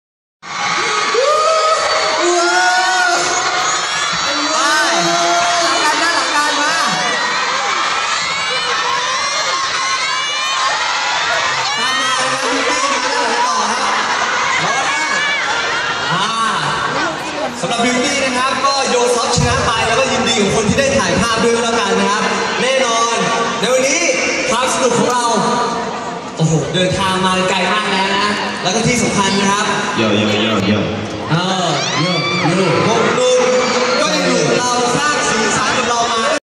150307 Beast Fan Meeting Bangkok - Walk to stage
Long time no see finally we met BEAST♥B2UTY